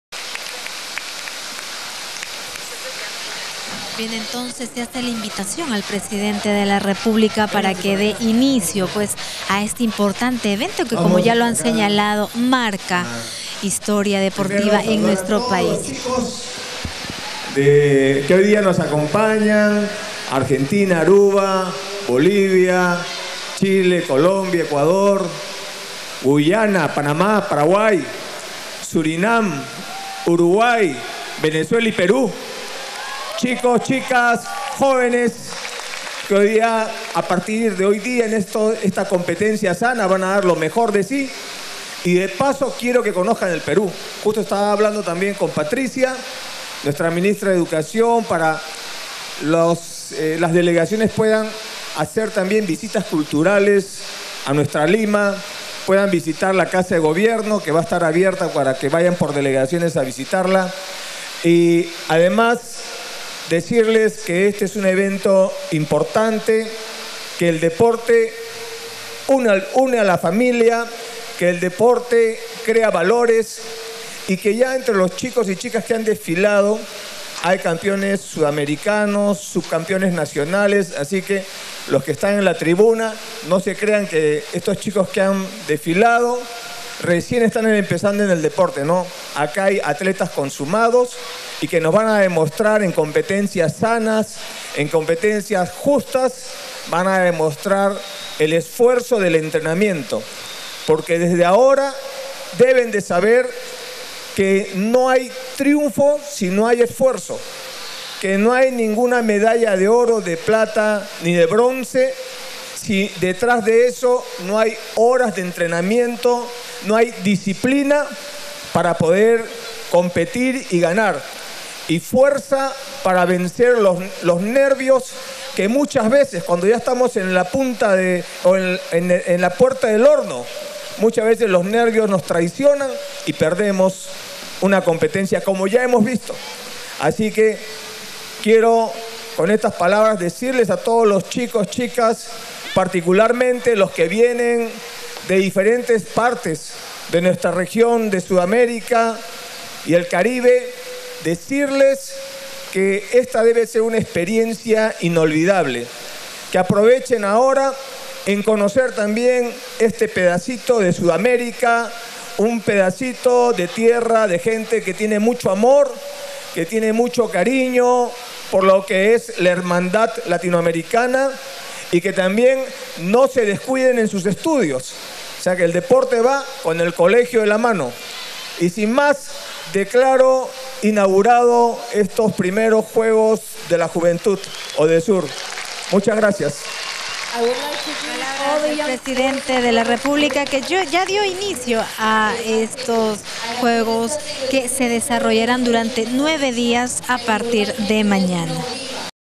Pdte Humala inauguró los Primeros Juegos Suramericanos de la Juventud
El Presidente de la República, Ollanta Humala Tasso inauguró esta noche los Primeros Juegos Suramericanos de la Juventud que se llevarán a cabo del 20 al 29 de setiembre en la ciudad de Lima y que permitirá clasificar a las Olimpiadas de la Juventud "Nanjing 2014". En este evento realizado en la Plaza de Armas, el Jefe de Estado advirtió que no hay triunfos, si no hay horas de entrenamiento y disciplina para competir y ganar. "Si no hay esfuerzos no hay ninguna medalla de oro, de plata, ni de bronce", acotó.